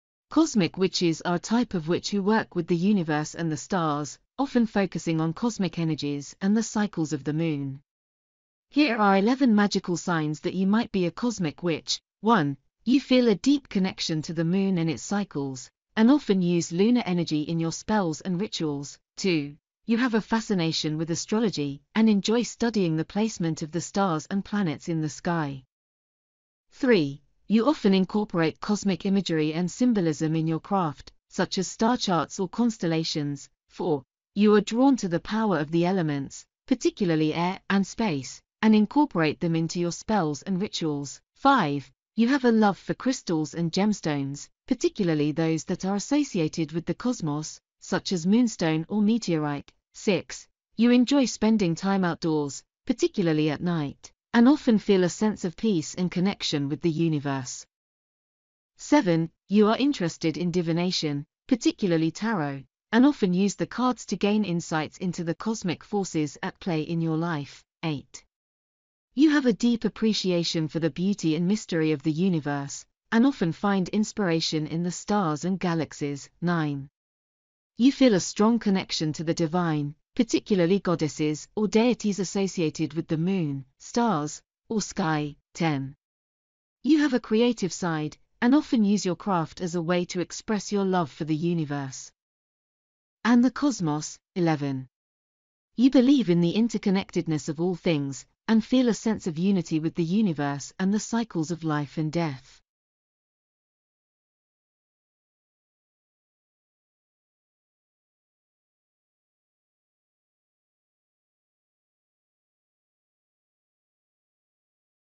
11 magical signs you might be a Cosmic Witch - Types of Witches Series
🔥 FREE BONUS. Charms, Spells and Herbs:
🔥 Your Soul Reading Reveals The Path That Your Soul Is On At This Point In Your Life

Fond of Astrology, Moon Magic, and Solar System? You might be a Cosmic Witch! Check this post for more cosmic insight when it comes to magic and alchemy,

Cosmic Witch loves astrology, zodiac, night sky and working with the Moon. Does this sound like you? If so, keep on reading and find out if Cosmic Witchery is a path for you

Cosmic Witches are a type of witch who work with the universe and the stars, often focusing on cosmic energies and the cycles of the moon. Here are 11 magical signs that you might be a Cosmic Witch:

1: You feel a deep connection to the moon and its cycles, and often use lunar energy in your spells and rituals.
2: You have a fascination with astrology, and enjoy studying the placement of the stars and planets in the sky.
3: You often incorporate cosmic imagery and symbolism in your craft, such as star charts or constellations.
4: You are drawn to the power of the elements, particularly air and space, and incorporate them into your spells and rituals.
5: You have a love for crystals and gemstones, particularly those that are associated with the cosmos, such as moonstone or meteorite.
6: You enjoy spending time outdoors, particularly at night, and often feel a sense of peace and connection with the universe.
7: You are interested in divination, particularly tarot, and often use the cards to gain insights into the cosmic forces at play in your life.
8: You have a deep appreciation for the beauty and mystery of the universe, and often find inspiration in the stars and galaxies.
9: You feel a strong connection to the divine, particularly goddesses or deities associated with the moon, stars, or sky.
10: You have a creative side, and often use your craft as a way to express your love for the universe and the cosmos.
11: You believe in the interconnectedness of all things, and feel a sense of unity with the universe and the cycles of life and death.